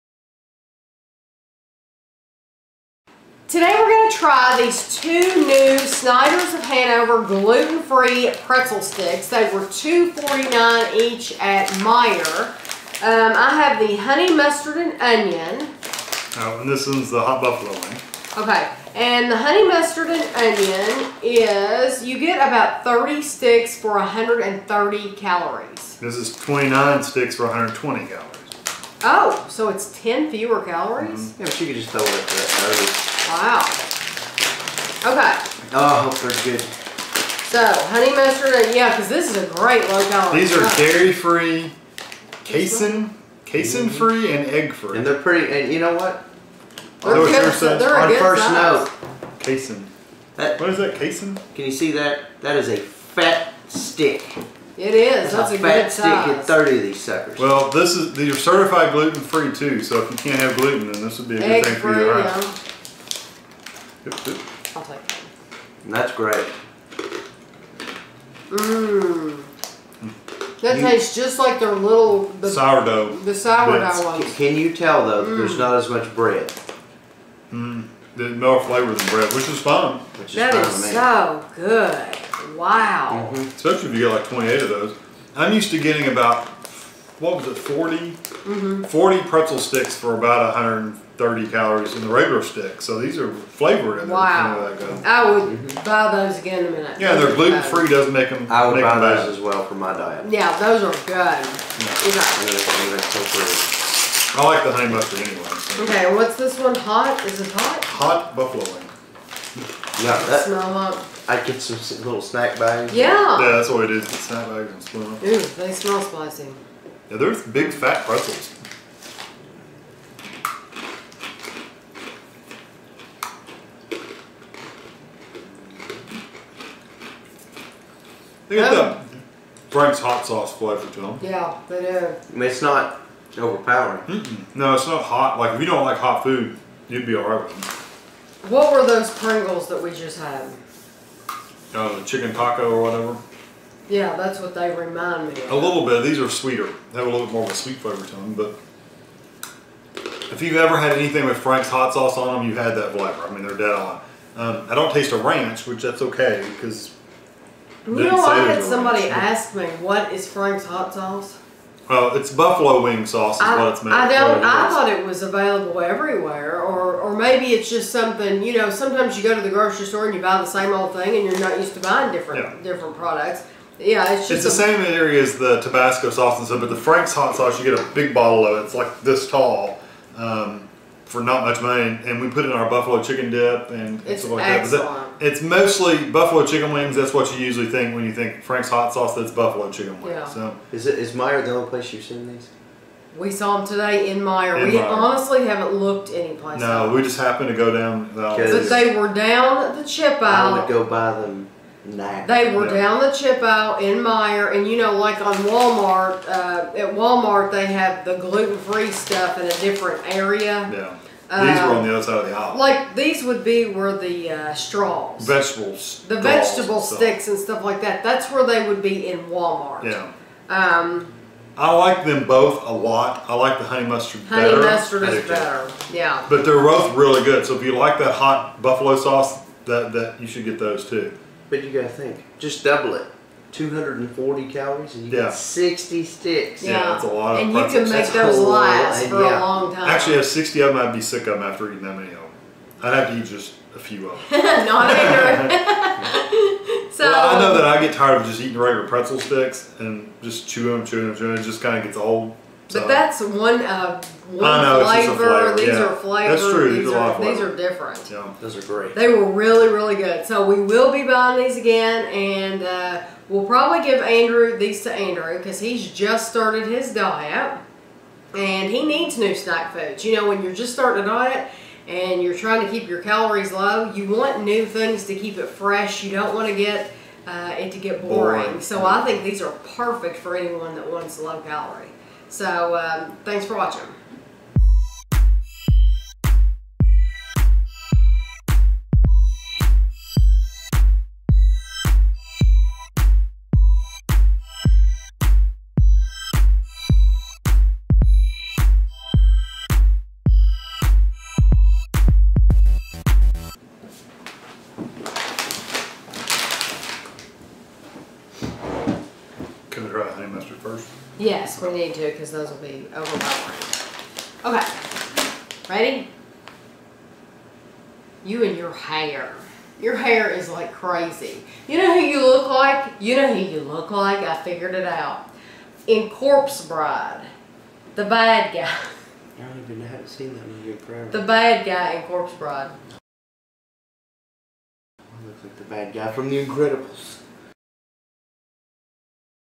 Snyder's of Hanover: Gluten Free Honey Mustard & Onion and Hot Buffalo Wing Review
This is a taste test/review of the Snyder's of Hanover Gluten Free Honey Mustard & Onion and Hot Buffalo Wing Pretzel Sticks. They are dairy free, casein free, gluten free and egg free. They were $2.49 per bag at Meijer. The Honey Mustard & Onion are 130 calories for 30 sticks. The Hot Buffalo Wing are 120 calories for 29 sticks.